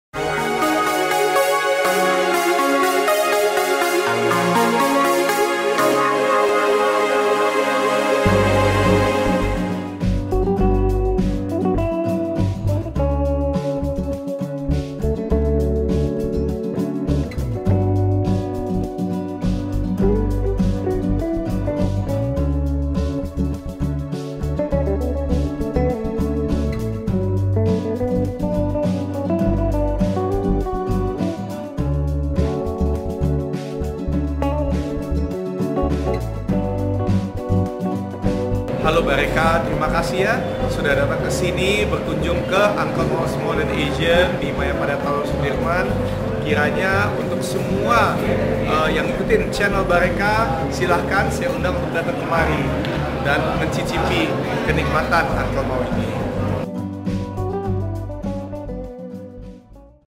Uncle Mao with Ari Wibowo on BARECA Choice
Dear all of you who enjoy good food! Let's hear more from Ari Wibowo about his culinary business! Grab your magazine now to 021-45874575 / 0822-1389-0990 :D